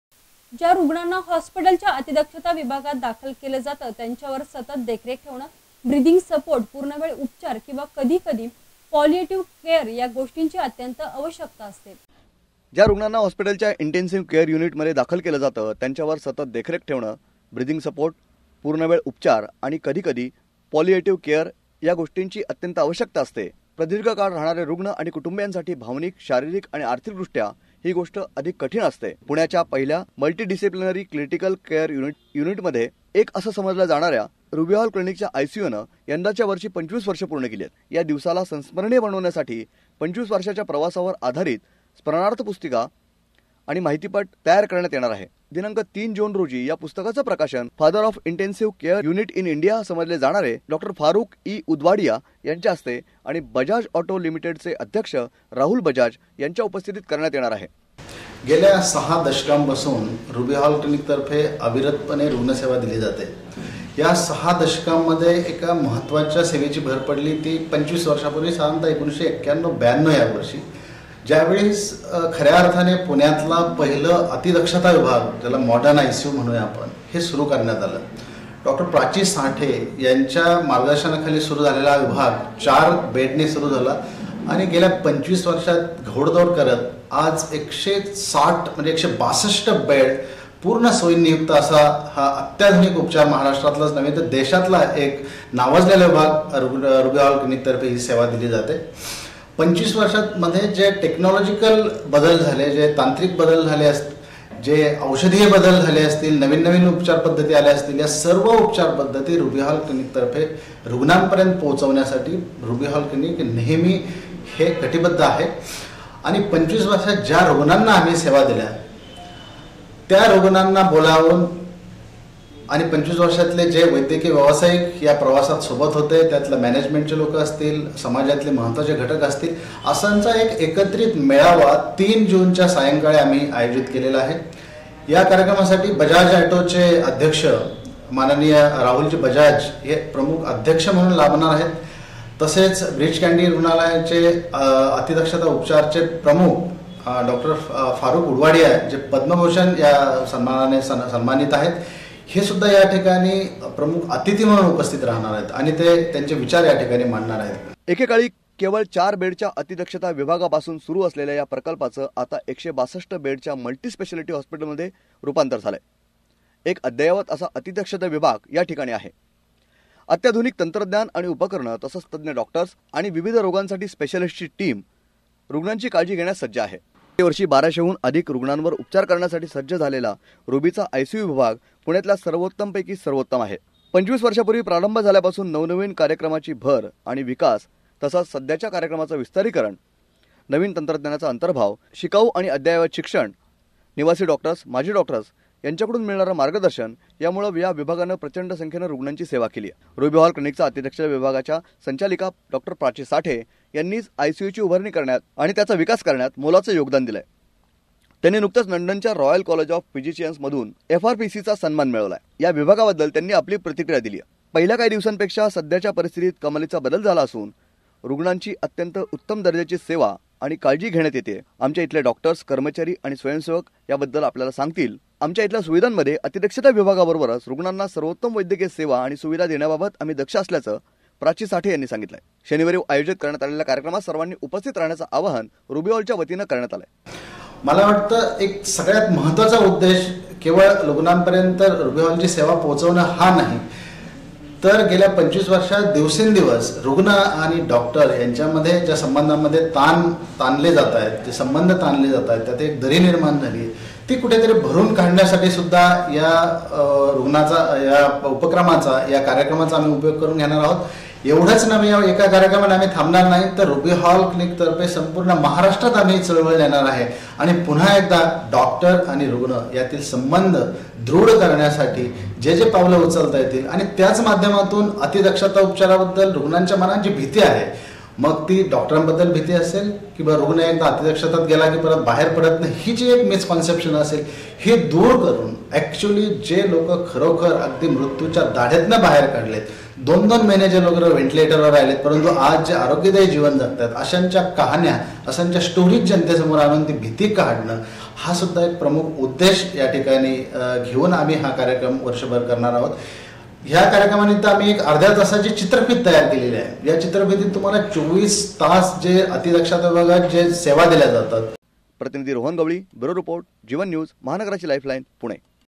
रूबी हॉल क्लिनिकच्या आयसीयु ला २५ वर्षे पूर्ण
ज्या रूग्णांना हॉस्पिटलच्या अतिदक्षता विभाग दाखल केले जाते त्यांच्यावर सतत देखरेख ठेवणे,ब्रिदींग सपोर्ट,पूर्ण वेळ उपचार किंवा कधी कधी पॅलिएटिव्ह केअर या गोष्टींची अत्यंत आवश्यकता असते. प्रदीर्घ काळ राहणारे रूग्ण व कुटुंबियांसाठी भावनिक,शारीरिक आणि आर्थिकदृष्ट्या ही गोष्ट अधिक कठिण असते.पुण्याचे पहिल्या मल्टीडिसिप्लिनरी क्रिटिकल केअर युनिटस मध्ये एक असे समजले जाणार्याण रूबी हॉल क्लिनिकच्या आयसीयुने (अतिदक्षता विभाग) यंदाच्या वर्षी 25 वर्षे पूर्ण केली आहेत.या दिवसाला अधिक संस्मरणीय बनविण्यासाठी या 25 वर्षाच्या प्रवासावर आधारित स्मरणार्थ पुस्तिका व माहितीपट सादर करण्यात येणार आहे. शनिवार,दि.3 जून 2017 रोजी या पुस्तकाचे प्रकाशन प्रमुख अतिथी फादर ऑफ इंटेंसिव्ह केअर युनिट इन इंडिया समजले जाणारे डॉ.फारोख ई.उदवाडिया यांच्या हस्ते व बजाज ऑटो लि.चे अध्यक्ष राहुल बजाज यांच्या उपस्थितीत करण्यात येणार आहे.